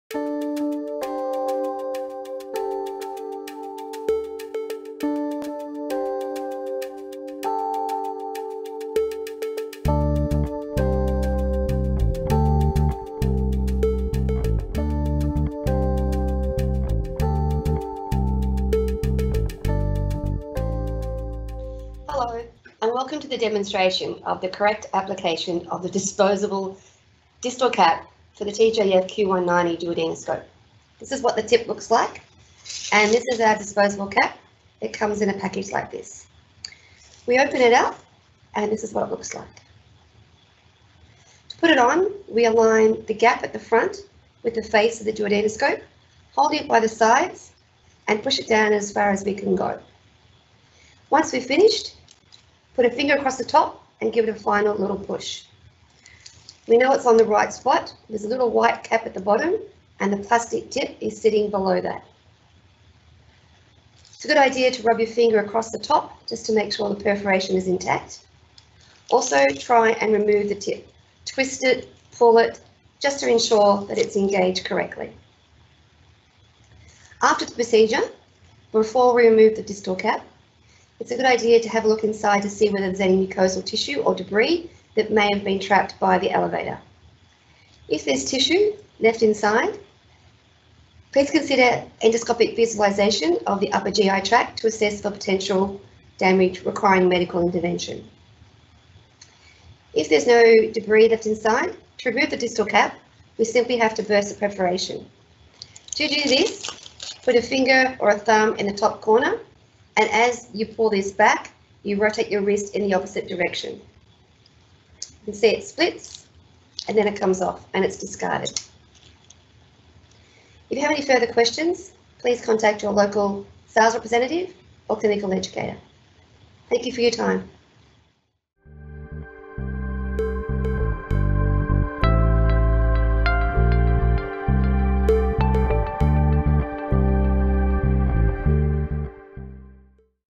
Olympus Academy - How to use the MAJ-2315 Disposable Distal Cap with the TJF-Q190V Duodenoscope
An in-service overviewing the use of the MAJ-2315 disposable distal cap with the TJF-Q190V duodenoscope.